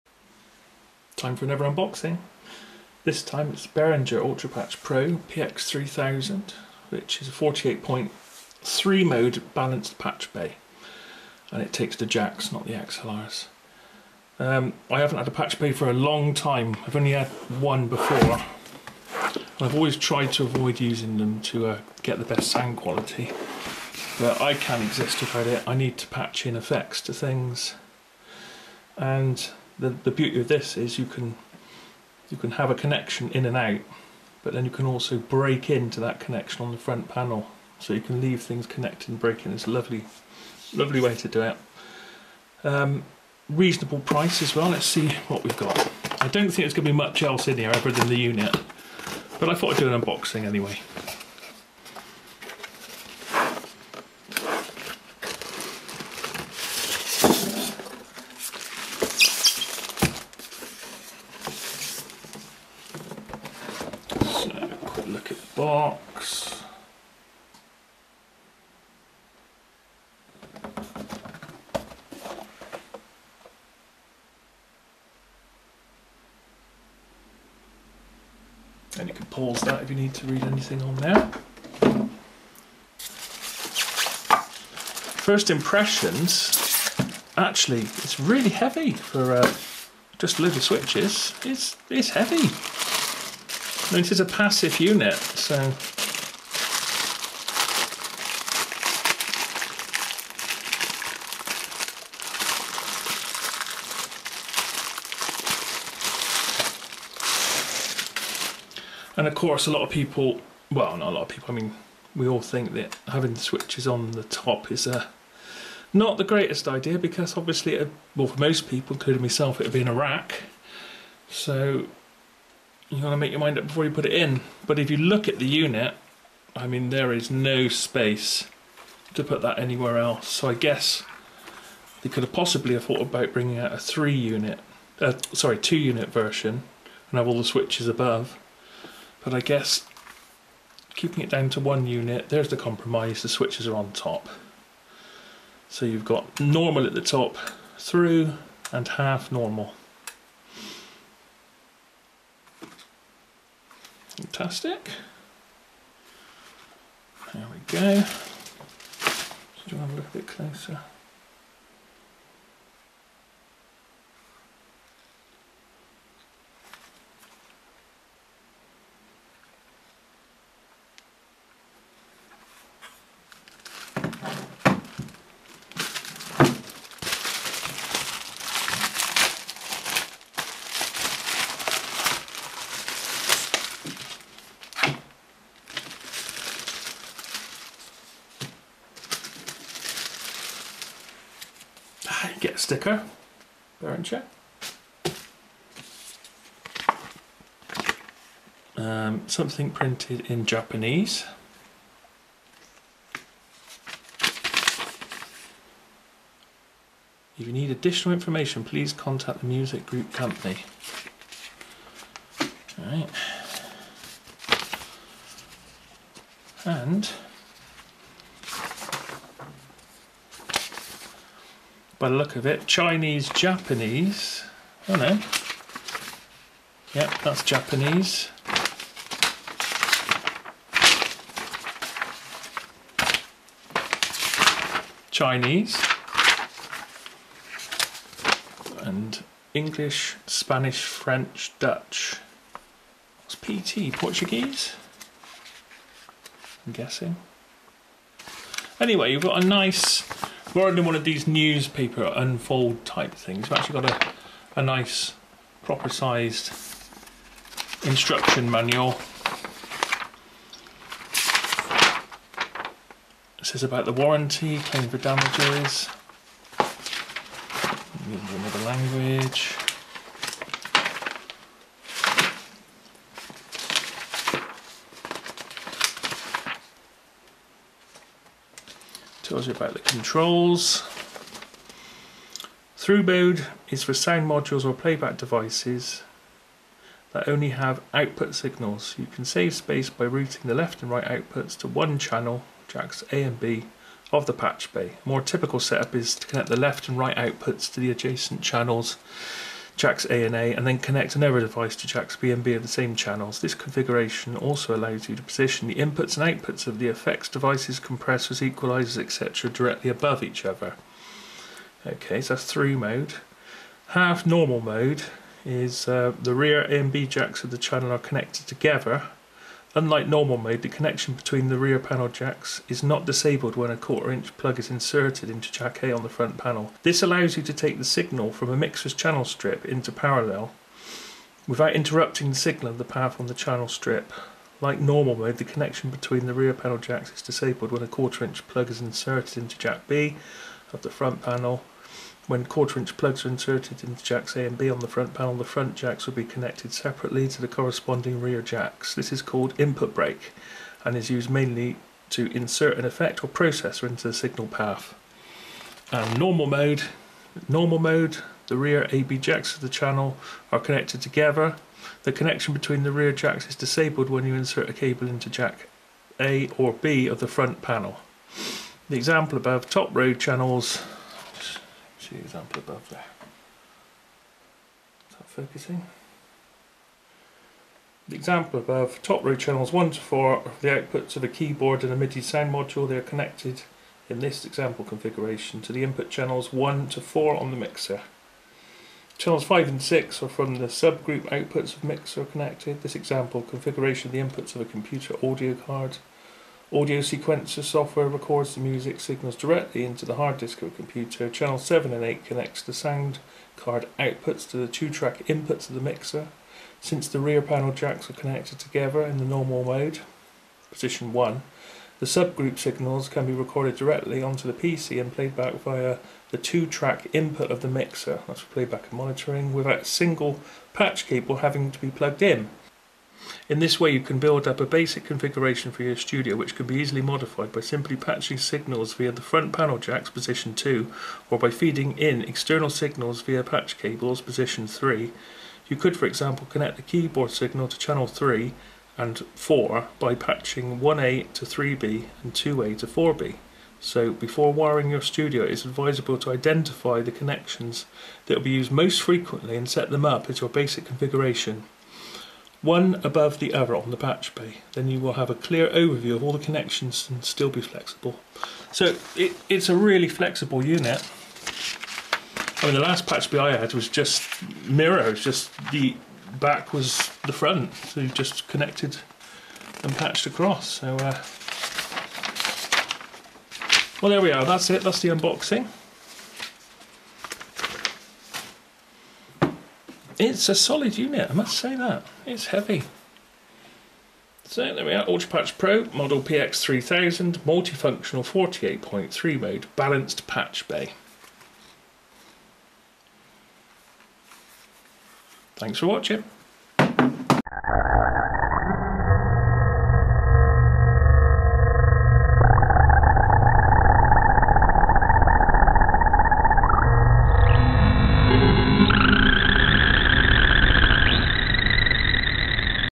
Behringer ULTRAPATCH PRO PX3000 Unboxing
ULTRAPATCH PRO PX3000
Multi-Functional 48-Point 3-Mode Balanced Patchbay
Professional patchbay eliminates cable clutter in your studio and adding order and clarity
Fully balanced high-quality ¼" TRS connectors
3 modes, easily selectable per channel via top-side switches
“Normal” interconnects the two rear jacks of one channel; inserting a plug into one of the front jacks interrupts the connection of the rear jacks
“Thru” interconnects each rear jack with its corresponding front jack
“Half Normal” interconnects the two rear jacks of one channel; inserting a plug into the lower front jack interrupts the connection of the rear jacks.